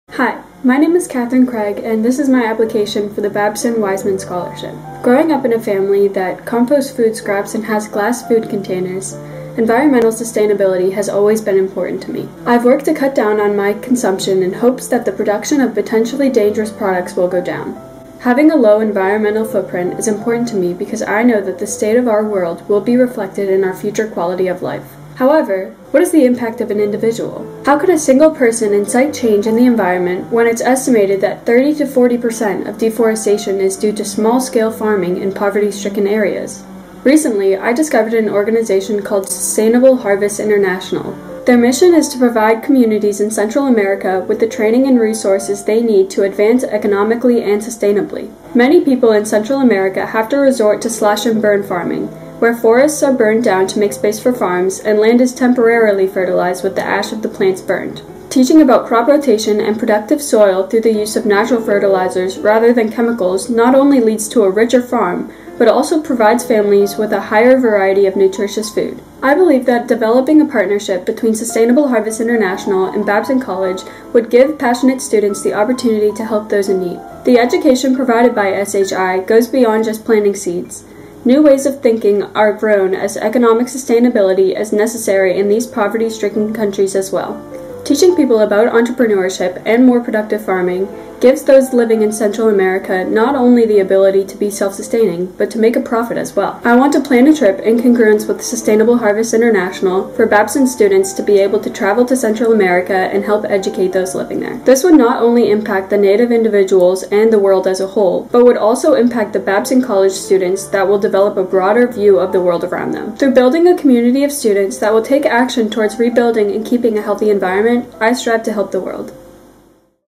Babson College Weissman Scholarship Application - Kathryn Craig
My video application for the Babson College Weissman Scholarship.